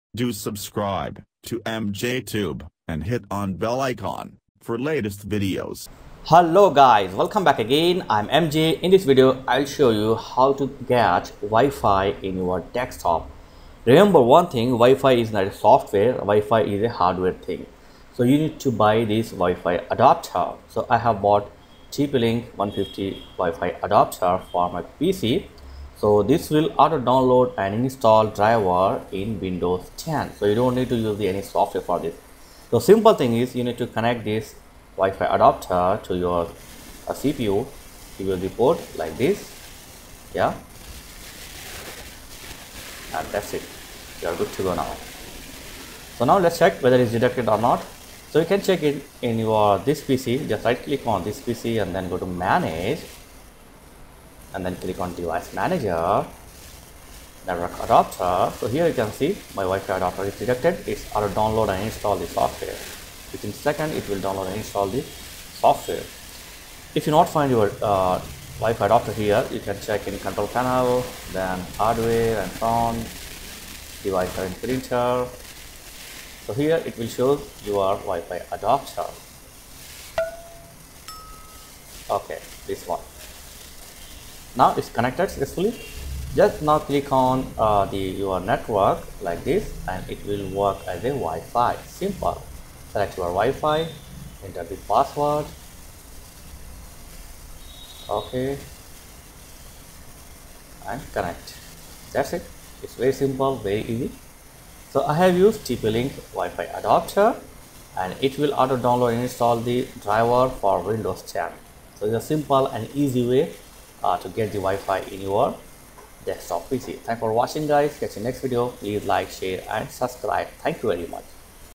How to Get Wi-Fi for Desktop PC In Windows 10 (No Software Needed)
Enable Wi-Fi in Windows 10 Desktop PC